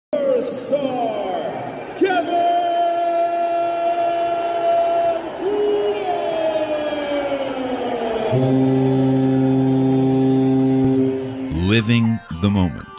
Veteran leadership💪🏒
Kevin Poulin veteran leader of the Laval Rocket named first star. Part of full video story for his contribution to get his team back on track after a tough start to the season.